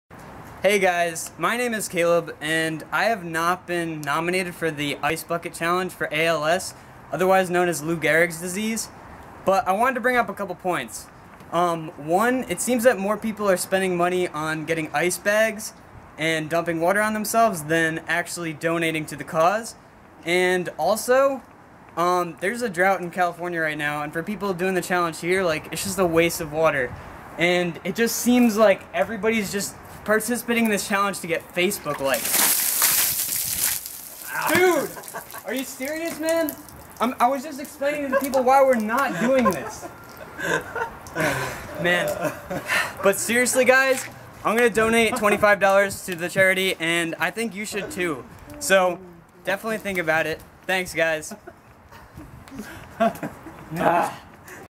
Caleb on the ALS Ice Bucket Challenge
Luckily I was conveniently under a tree that needed water anyway!